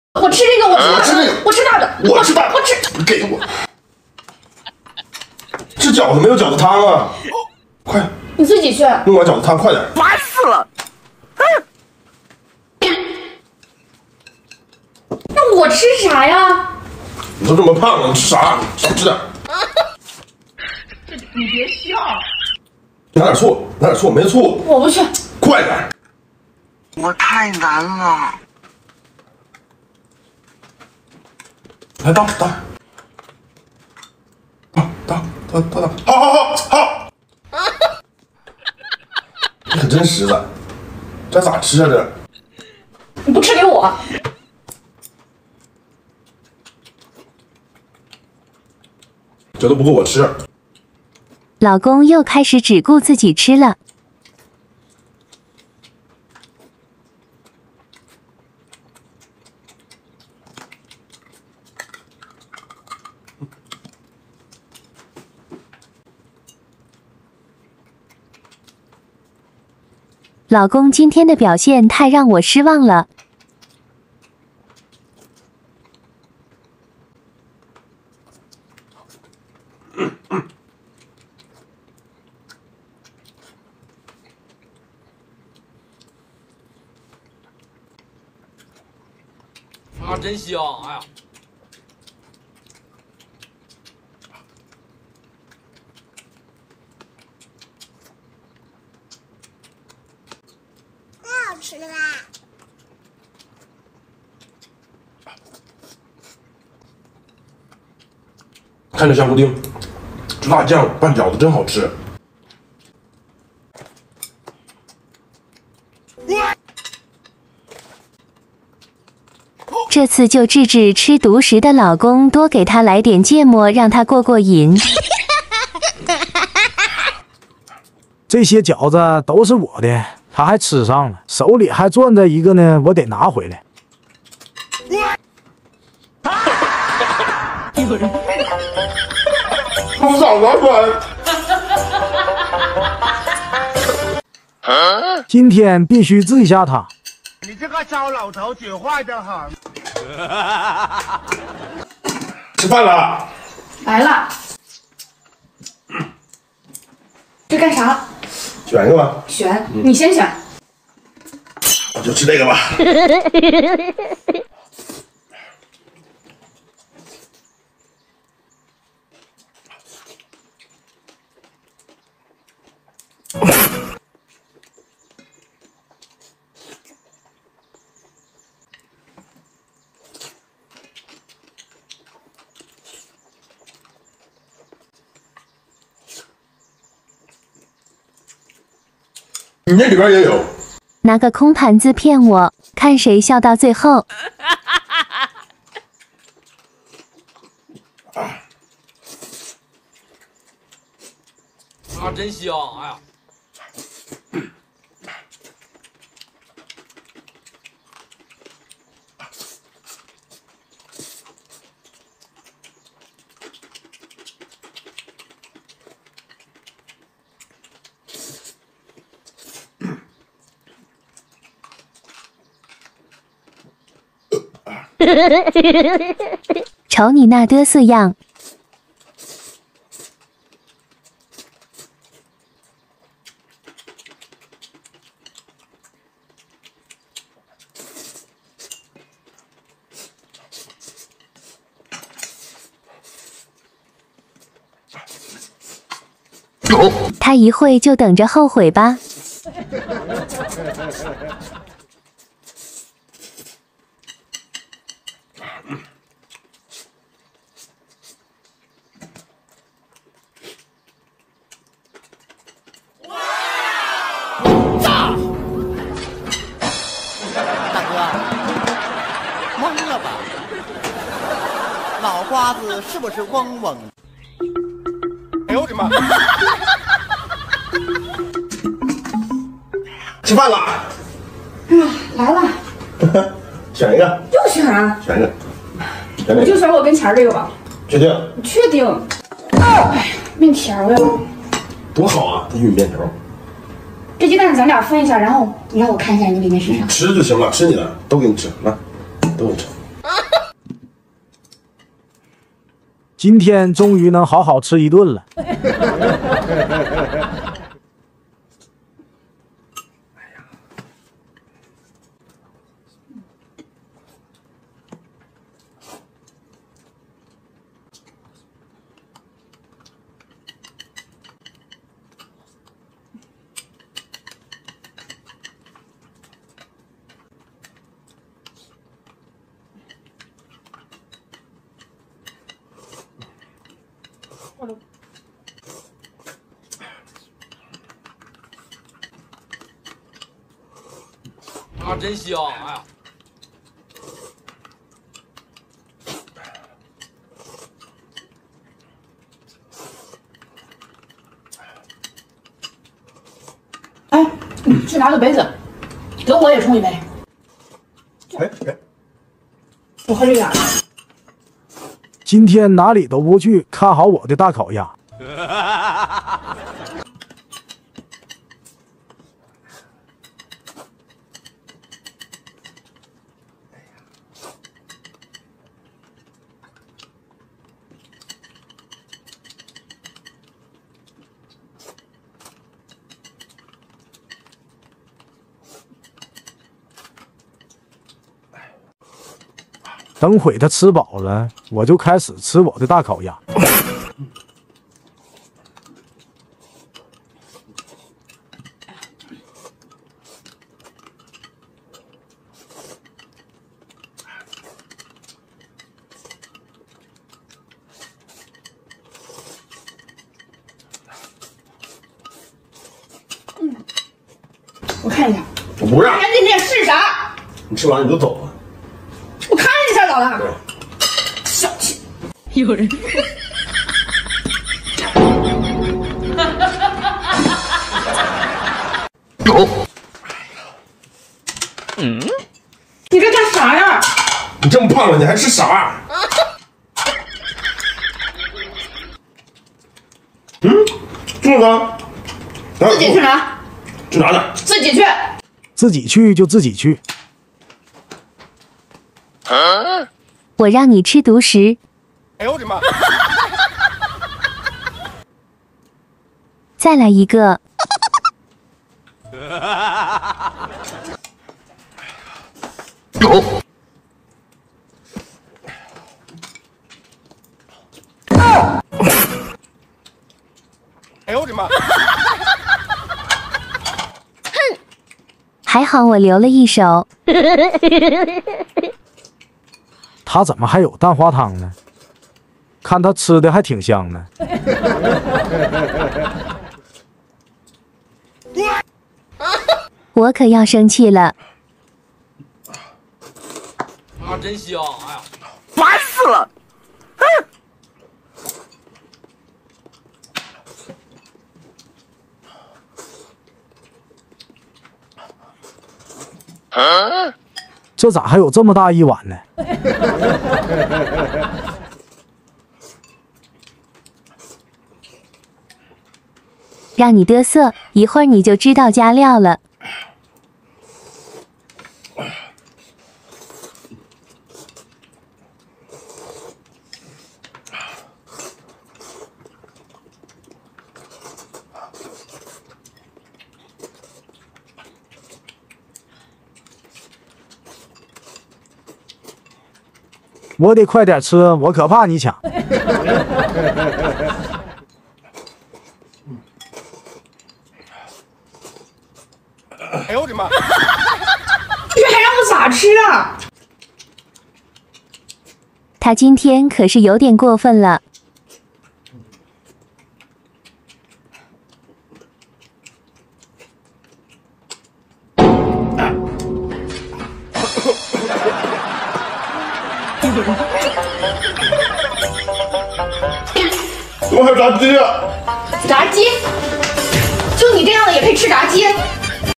老公一个人把两份饺子全吃了，先放点芥末整他，在拿出烧鸡来自己吃！【王小妮爱美食】
大家好！ 这是王小妮爱美食😍
🍜更新搞笑夫妇斗智斗勇的日常生活
🍤分享分享美味又实惠的美食
带你吃遍世界各地的美食
希望它给你带来快乐